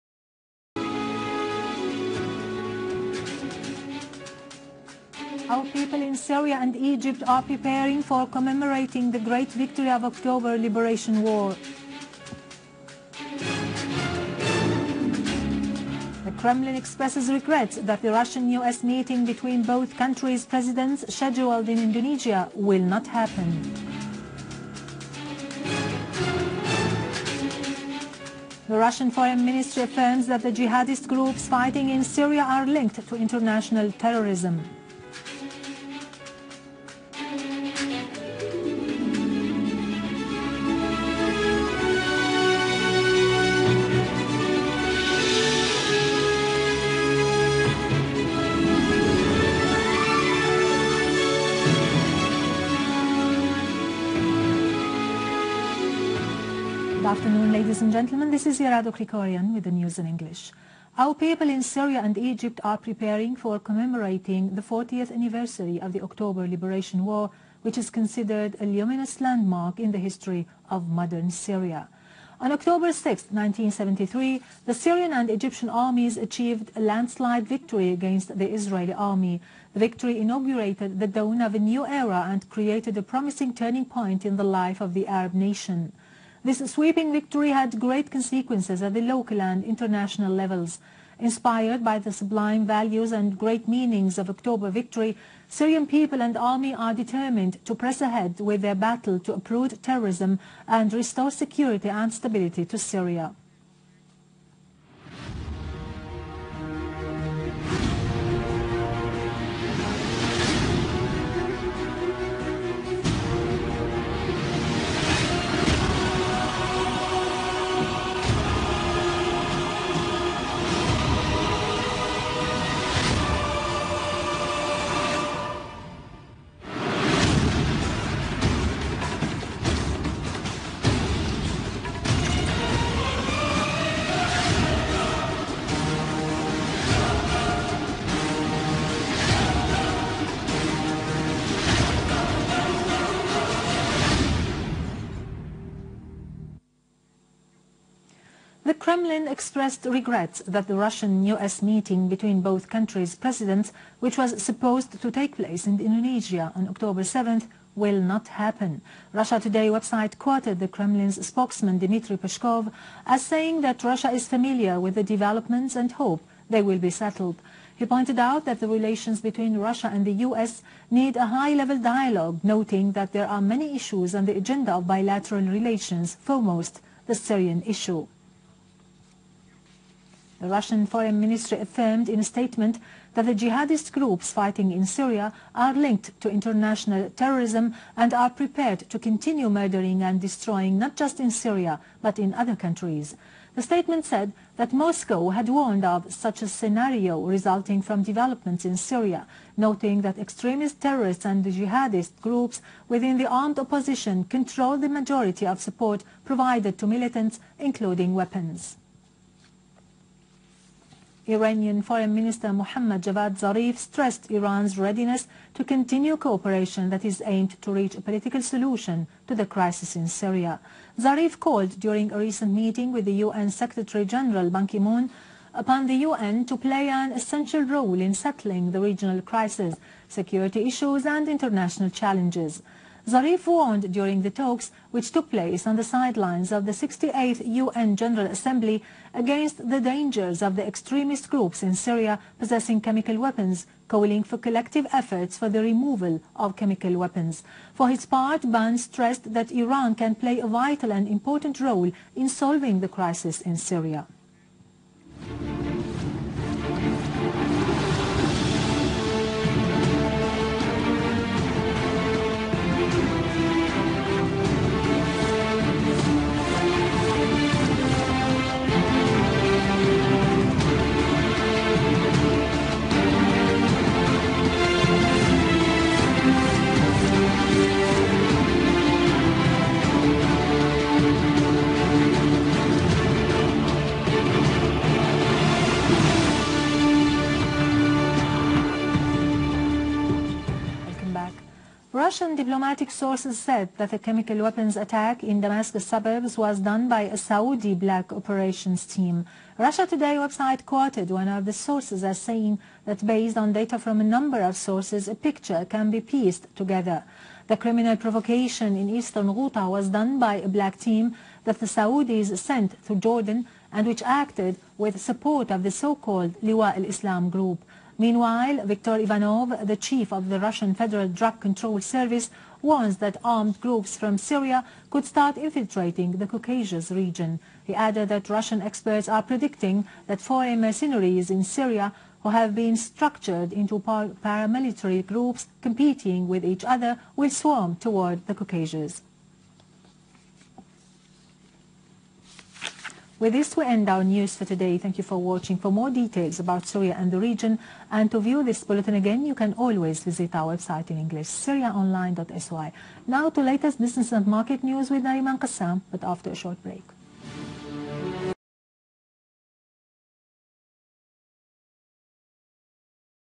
Syria News October 5, 2013
- Octrober War: Turning point in life of Arab nation
- Russia: Many issues particularly Syria need high-level Russia-US dialogue
- Russian FM: Jihadist groups in Syria linked international terrorism
- Tehran: Iran ready to continue cooperation to solve crisis in Syria politically

FOR THE TRUTH, FOR SYRIA
Video-Evidence of the Conspiracy Against Syria
... ... ...and associated web spaces, Youtube channels and Facebook pages... ... ...

Source:

Name: Syrian TV Official